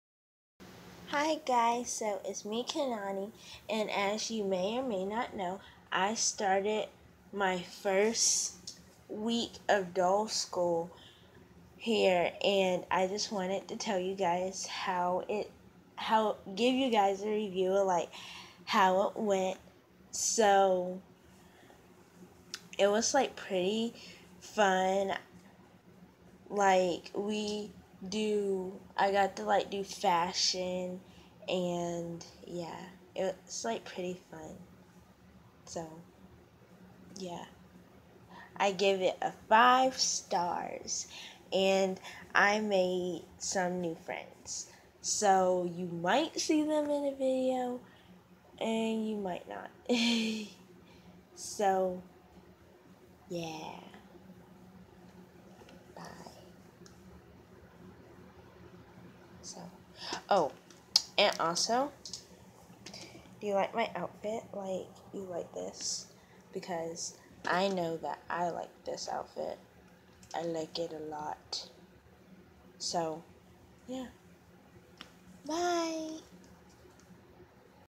Kanani's review of school
Short video but the next one will be longer we promise. Kanani liked her first week of school she cant wait to bring you to school after her Christmas break.